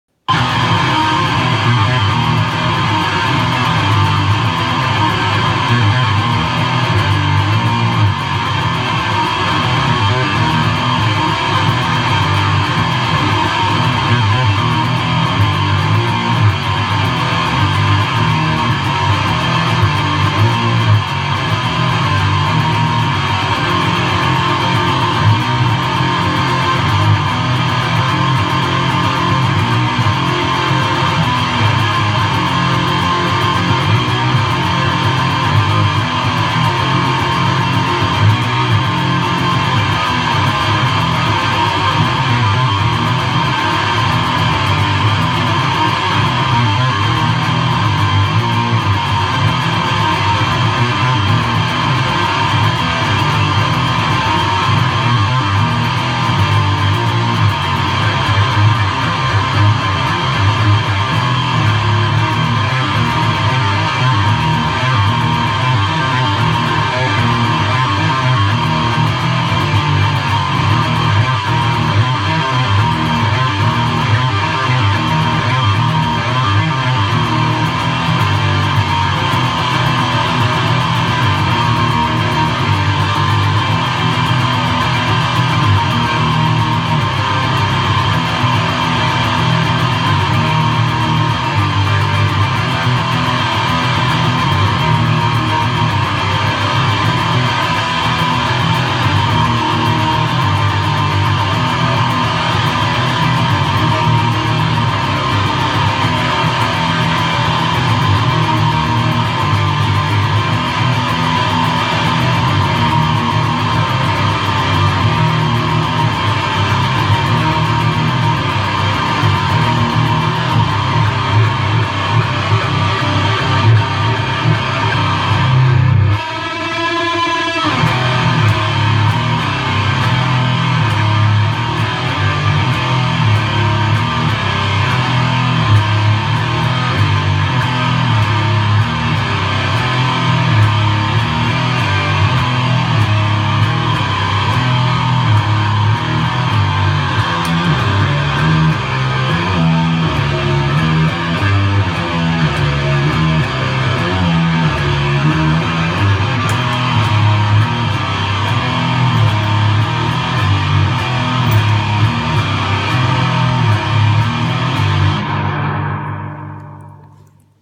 Untitled Punk song
LTD EC series into Abominable Electronics Throne Torcher and Earthquaker Ghost Echo through solid state Fender Mustang IV combo amp.